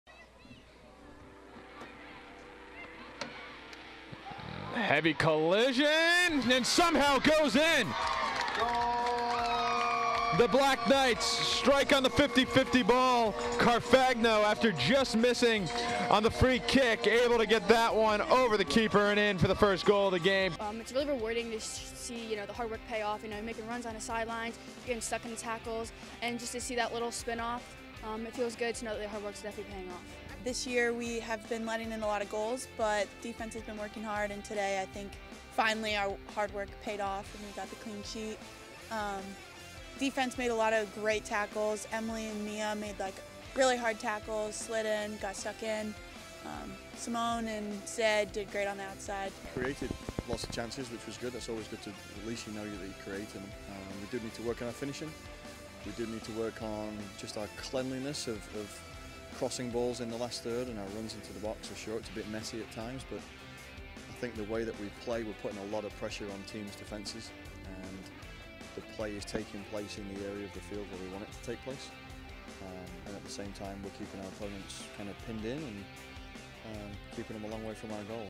Recap: Army Women's Soccer vs. Marist 9-18-19
Army defeated Marist 1-0 to end their non-conference schedule.